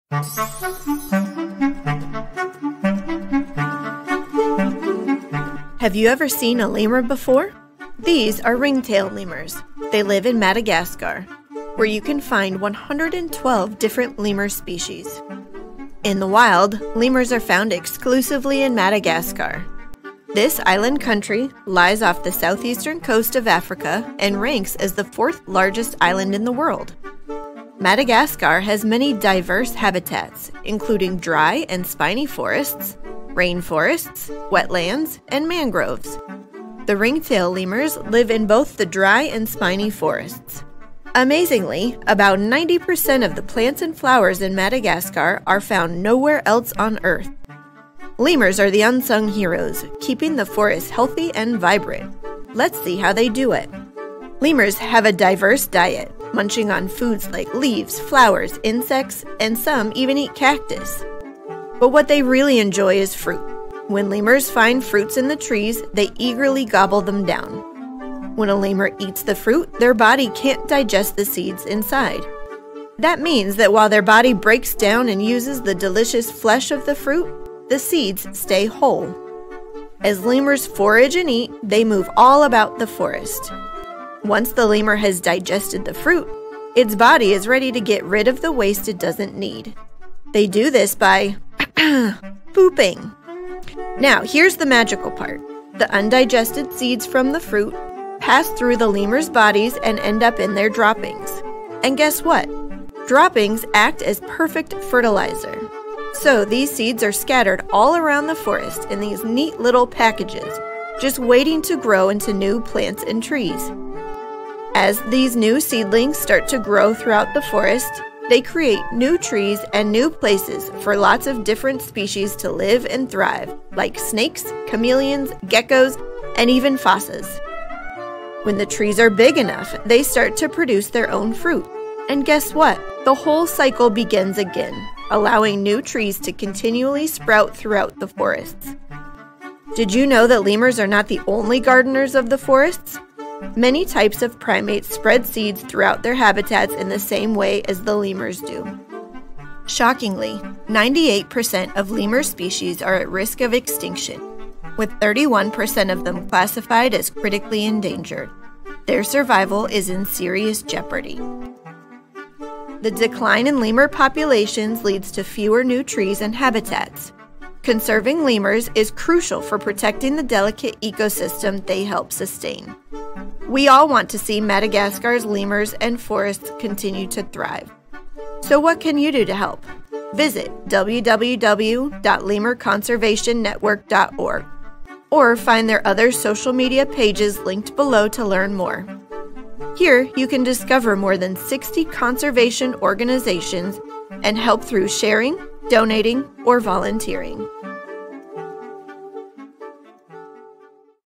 Lemurs: Gardeners of the Forest in Madagascar
Learn how lemurs and other primates help the forest grow through seed dispersal! This animated video is great for adults and kids of all ages to learn about this scientific process. The video illustrates how important lemurs and primates are for conserving forests and biodiversity in Madagascar, and explains how primates help forests grow around the world.

This video was created by our science communications volunteer, Tif Lester. Thank you, Tif!